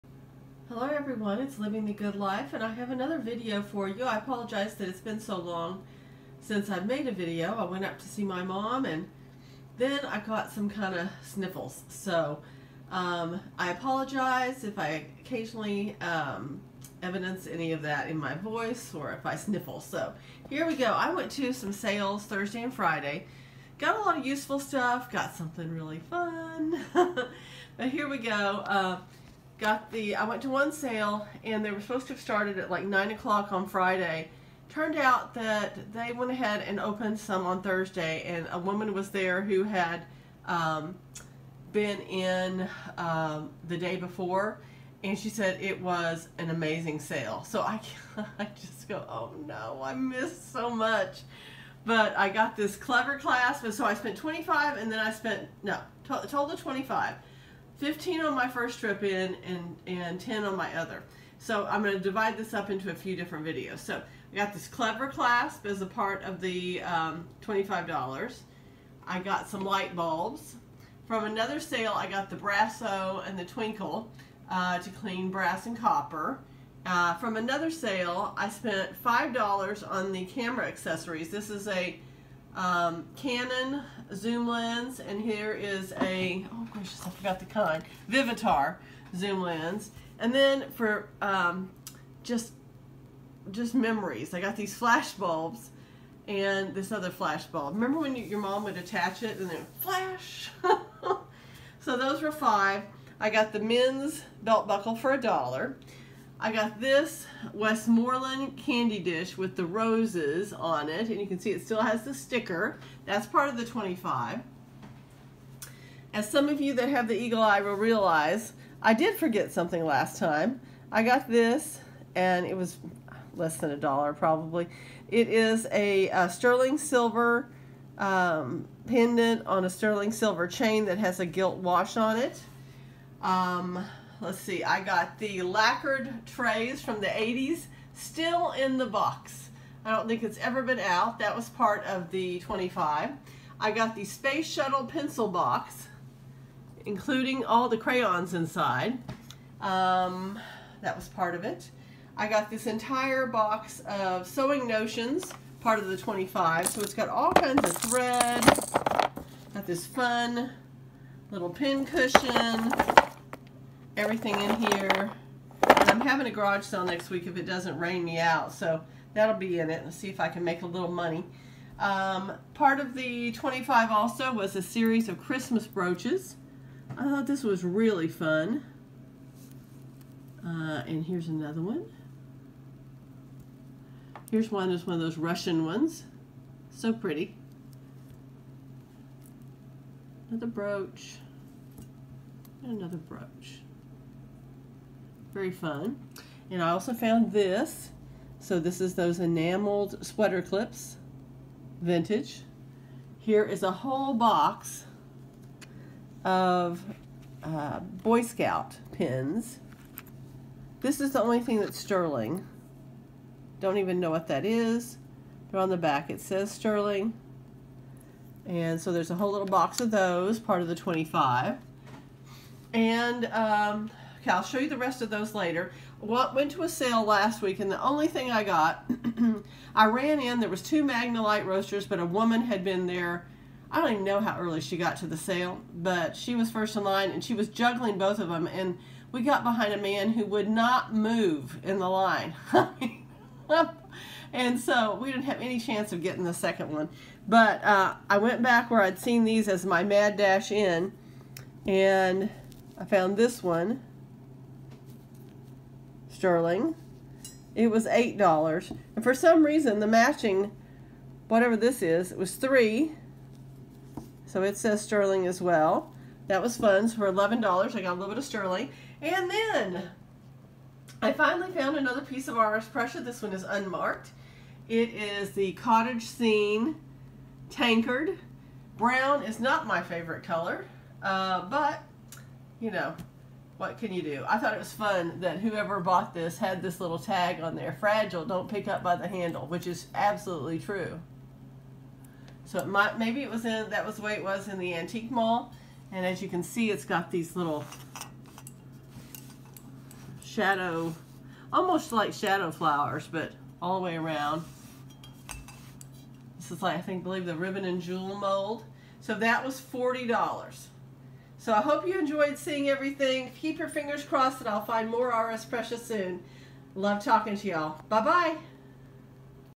Estate Sale Finds Video #378: RS Prussia, Jewelry, Sterling & More
This video is the beginning of a 2 or 3 part series from this past weeks finds.  Hope that you enjoy seeing all my finds.